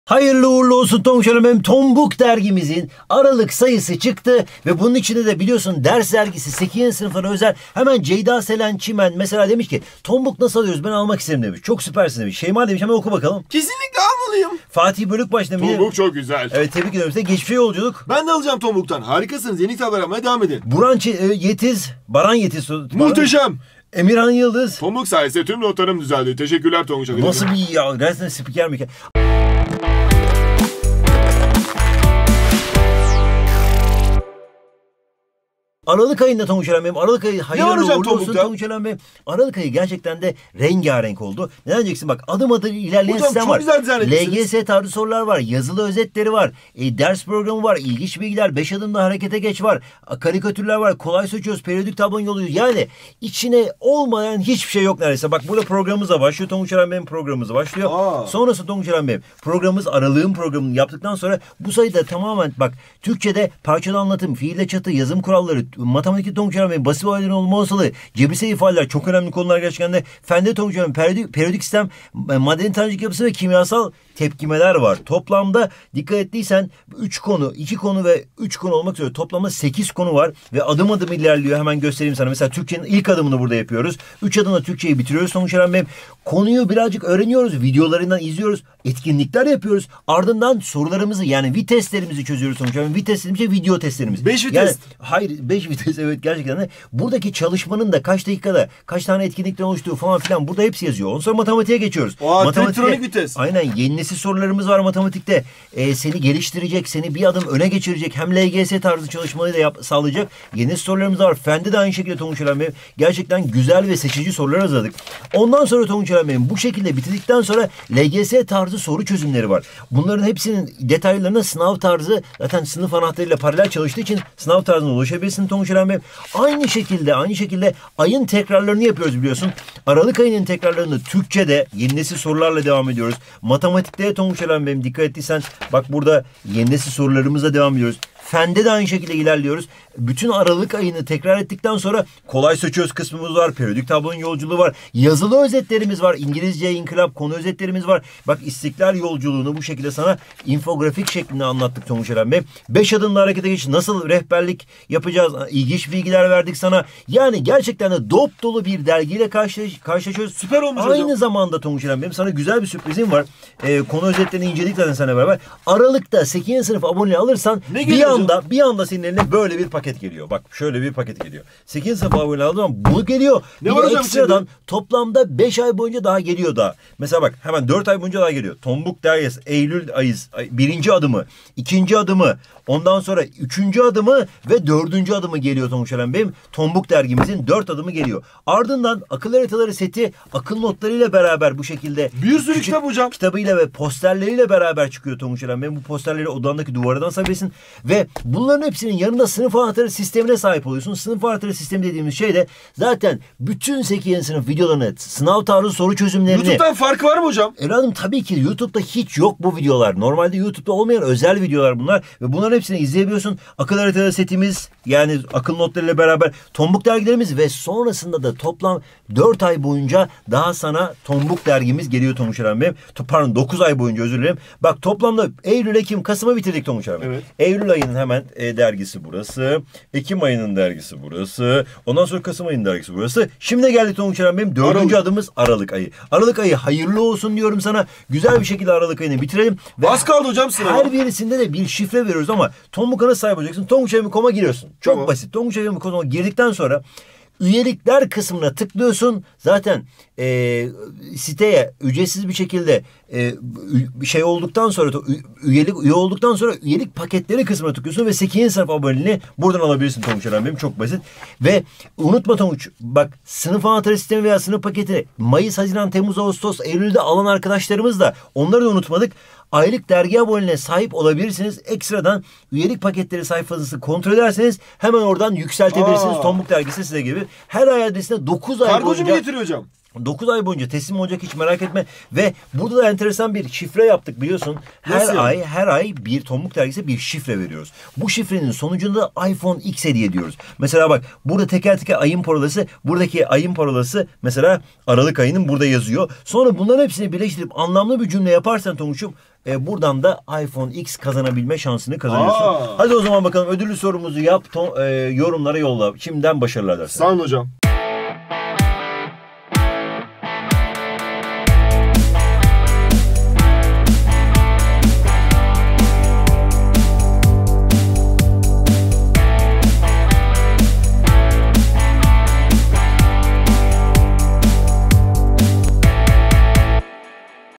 8.Sınıflara Özel Tonbook Ders Dergisi Aralık Sayısı ÇIKTI!
8.Sınıflara Özel Tonbook (Ders Dergisi) Aralık Sayısı ÇIKTI! 📢
Detaylı bilgi ve üyelik paketleri için Tonbook Ders Dergisi'ne buradan ulaşabilirsin
Peki Aralık sayısında neler var?
📌Adım adım ilerleme
📌LGS tarzı sorular
📌Yazılı özetleri
📌Ders programı
📌İlginç bilgiler
📌5 Adımda hareket geç
📌Karikatür
📌Kolaysa Çöz
📌Periyodik tablonun yolculuğu
Bu sayıyı sakın kaçırma!📚